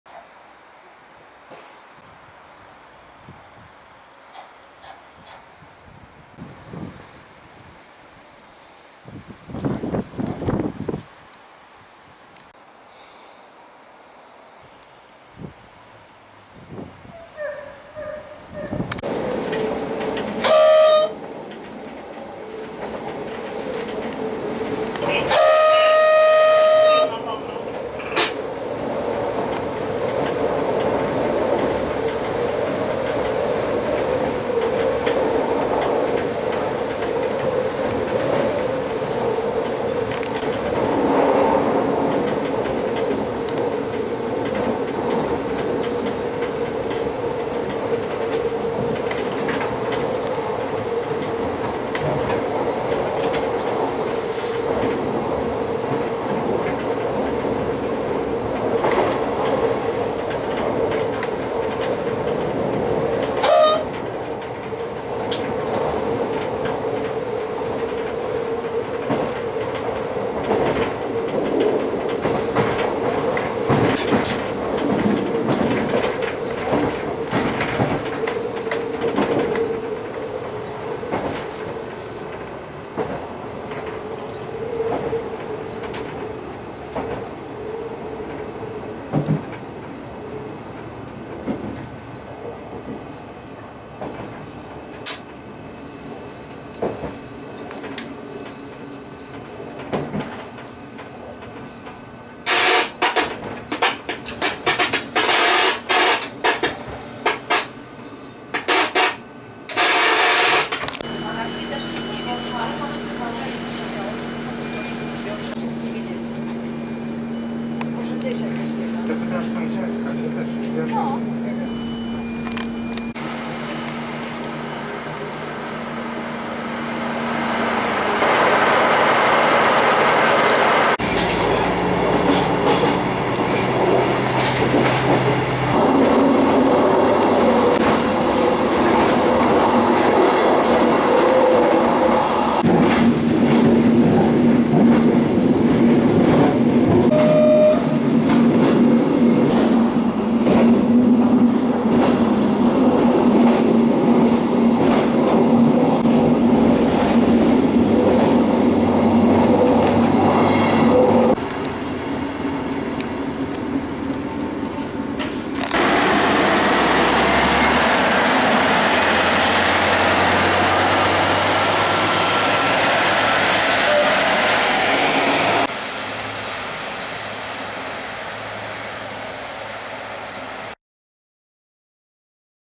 ARRIVA Kwidzyn - OSTATNIE DNI przewozów . movie
Jeden z ostatnich przejazdów Brachlewo - Kwidzyn , Kwidzyn - Brachlewo
ZWIASTUN DŁUŻSZEGO FILMU o pociągach ARRIVA